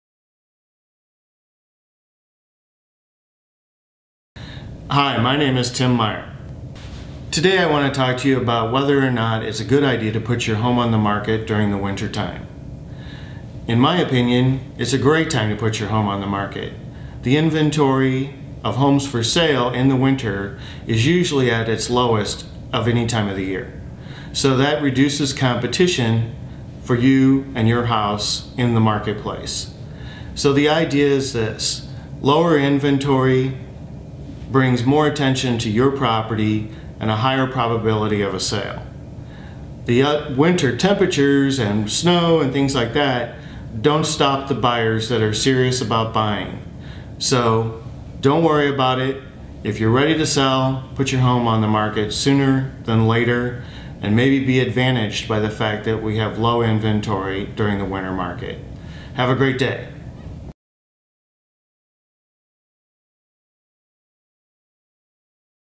Listing your home for sale in the winter
Are you waiting to list your house until warm weather is here to stay?   Winter is a great time to list your home.  Listen to Tim Meyer present this 1 minute video on the benefits of listing right now! Don't be concerned about the additional cold in the forecast.  Call us.  We can help.